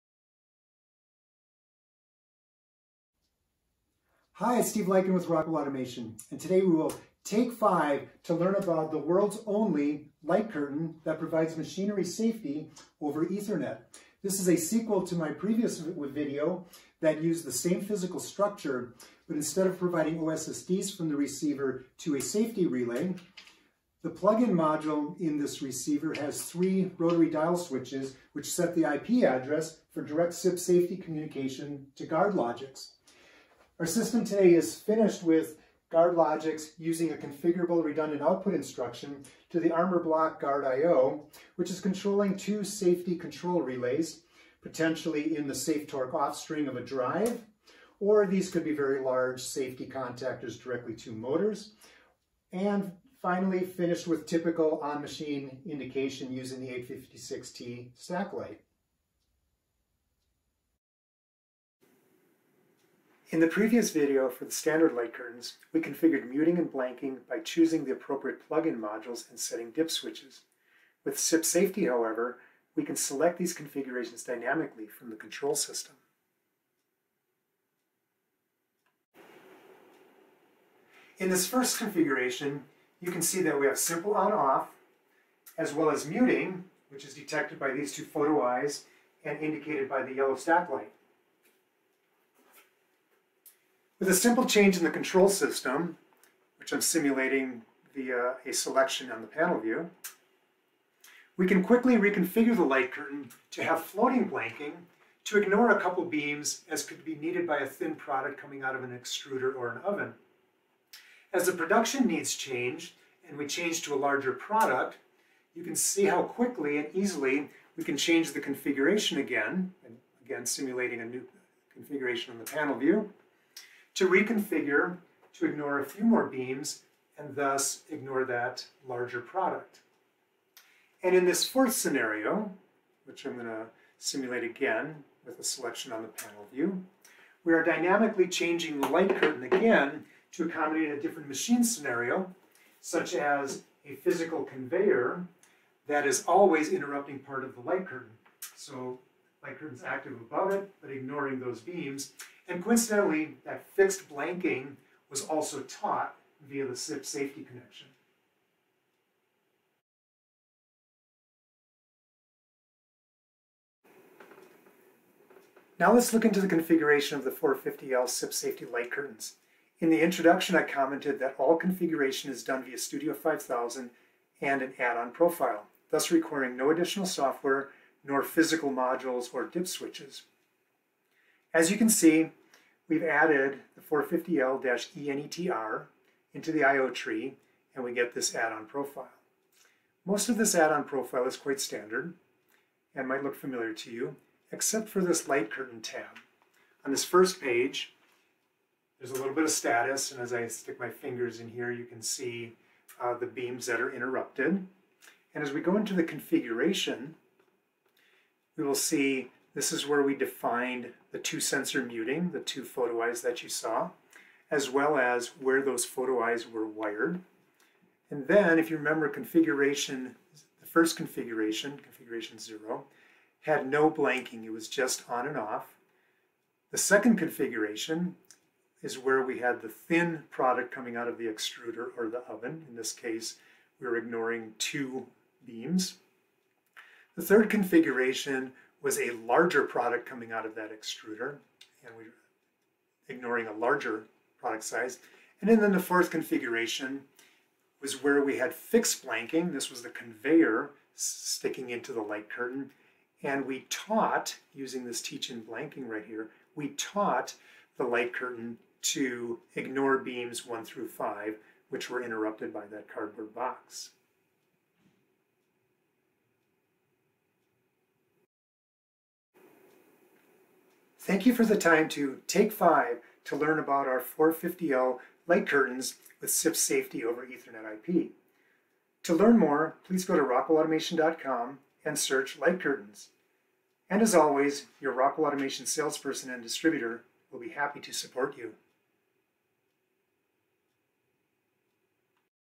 450L Sequel: Light Curtains with CIP Safety over Ethernet - Take5
Think outside the box and "Take5" to learn about the world's only light curtain that provides machinery safety using CIP Safety over Ethernet.

In this video we "Take5" to learn about integrating the 450L CIP Safety Light Curtains from Rockwell Automation into GuardLogix.

This is a sequel to the "450L Take5" which introduced the 450L Light Curtains but with standard plug-in modules instead of using CIP Safety with the 450L-APR-EN-8 plug-in module and the 450L-ENETR ethernet adapter.

Certified for use in safety applications up to and including SIL 3 / Performance Level E in compliance with applicable standards, these light curtains provide comprehensive functionality including:
Muting
Blanking
Cascading
Integrated Laser Alignment
Finger (14mm) and Hand (30mm) Resolution
Lengths from ~6" (150mm) to ~6' (1950mm)